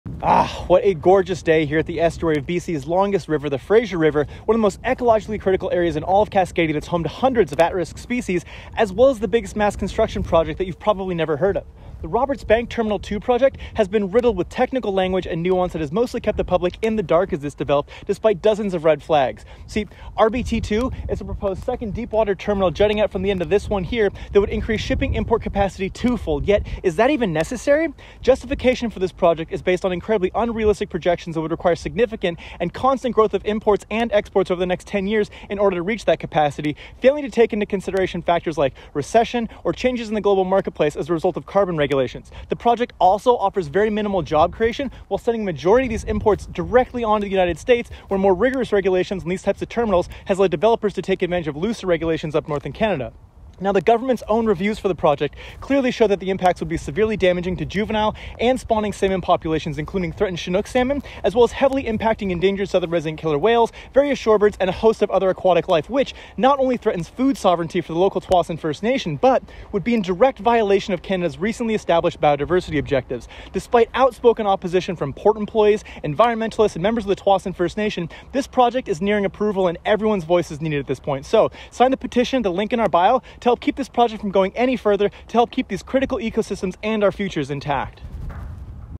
Problems with the Roberts Bank Terminal 2 Project
The estuary of the Fraser River is one of the most ecologically critical areas in Cascadia, home to hundreds of at risk species…as well as the biggest mass construction project you’ve never heard of. 😬

The Roberts Bank Terminal 2 project is riddled with technical language and nuance that has mostly kept the public in the dark as it has developed despite dozens of red flags.  A proposed second deep water terminal stemming from the end of the current Port of Vancouver shipping terminal, RBT2 would increase shipping import capacity twofold…yet, is it even necessary?

Justification for this development is based on incredibly unrealistic projections that would require significant and constant growth of imports and exports over the next 10 years to reach capacity, failing to take into consideration factors like recession or changes in the global marketplace as a result of carbon regulations.  The project also offers minimal job creation, while sending a majority of those imports directly on to the United States, where more rigorous regulations on these types of terminals has led developers to take advantage of looser ones up north in Canada.

The Government’s own reviews for the project clearly show that the impacts would be severely damaging to juvenile and spawning salmon populations including threatened Chinook Salmon, as well as heavily impacting endangered Southern Resident Killer Whales, various shorebirds and a host of other aquatic life, which not only threatens food sovereignty for the local Tsawwassen First Nation, but would be in direct violation of Canada’s  recently established biodiversity objectives.

Despite outspoken opposition from stakeholders such as port employees, environmentalists and members of the Tsawwassen First Nation, this project is nearing approval, but everyone’s voice is needed at this point!

If we reach 500 signatures on this legally binding petition, it’s required to be presented to the Minister of Environment and Climate Change, Steven Guilbeault, in the House of Commons.  Sign it today to help keep this project from going any further, to help keep these critical ecosystems and our futures intact!